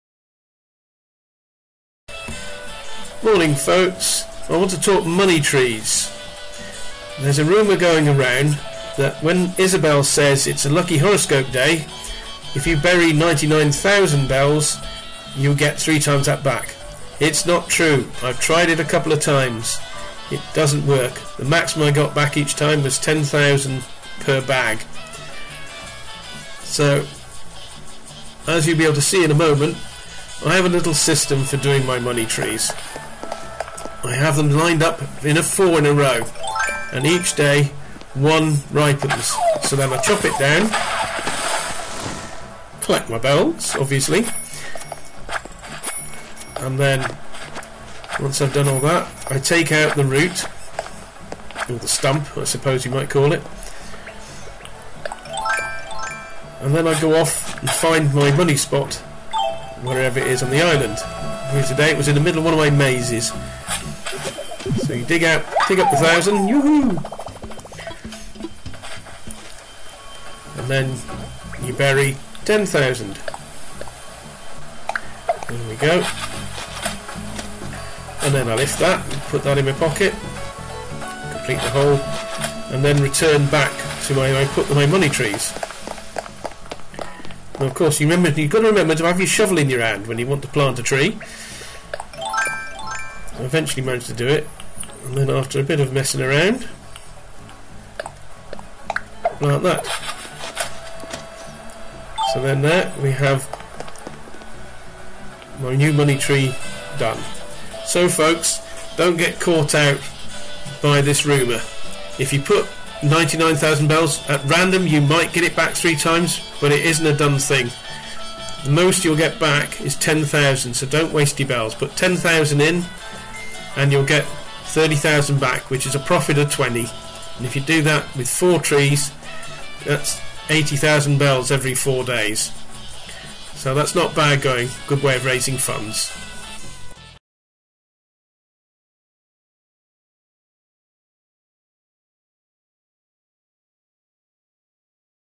MONEY TREES
a short film on how i do my money trees